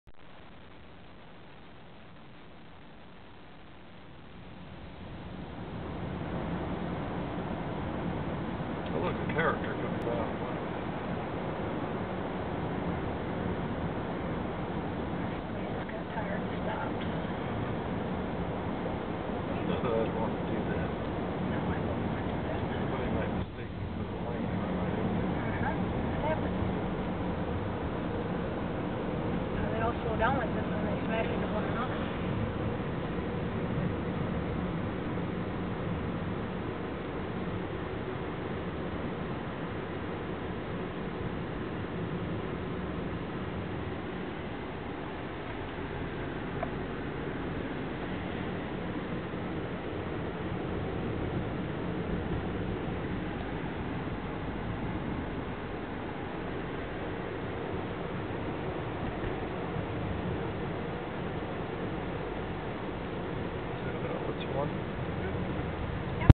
Snow on the NY State Thruway, 21 Dec 2008
25 MPH, 14 F, 6:02 PM